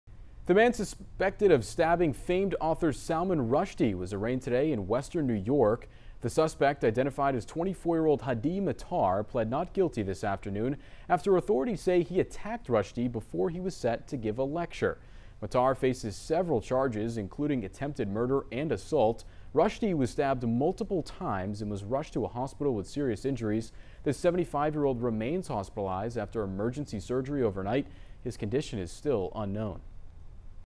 Man accused of stabbing Author Salman Rushdie arraigned in New York, pleads not guilty
He faces several charged including attempted murder and assault.